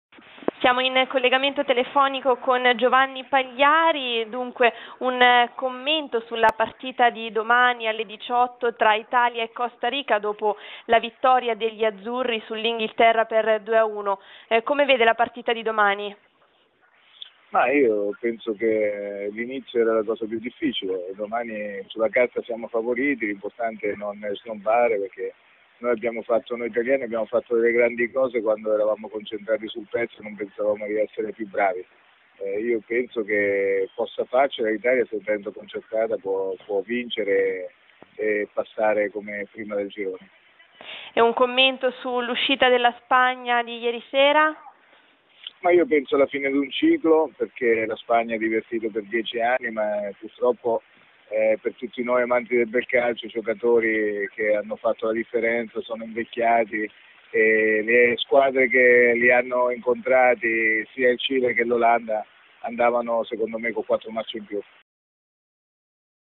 Mondiali 2014. Intervista a Giovanni Pagliari su Italia - Costa Rica
Dopo l'uscita di scena della Spagna di ieri sera che è caduta al cospetto del Cile, i riflettori sono puntati sulla seconda partita dell'Italia.
Questa volta gli azzurri saranno impegnati contro la Costa Rica, venerdì 20 giugno alle 18.00.